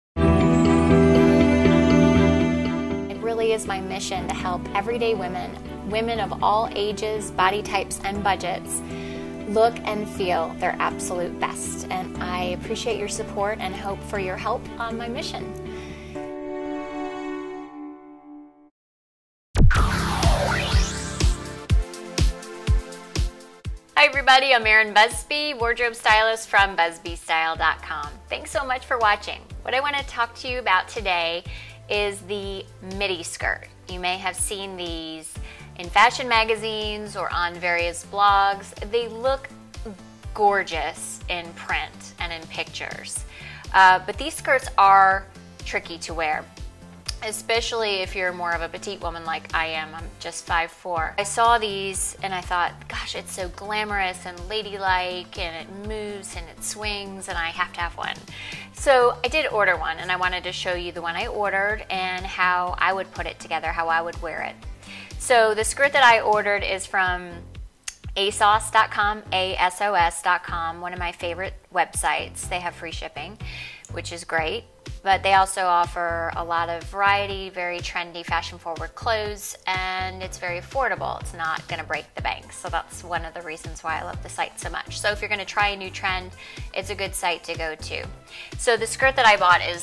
How To Wear A Midi Skirt
In this short video, I show you several tops you can pair with your full, midi skirt. I also show you some styling tricks to balance the volume of the full midi skirt to your advantage...helping you achieve that coveted hourglass body shape!

BusbeeStyle.com... Everyday Women. Extraordinary Style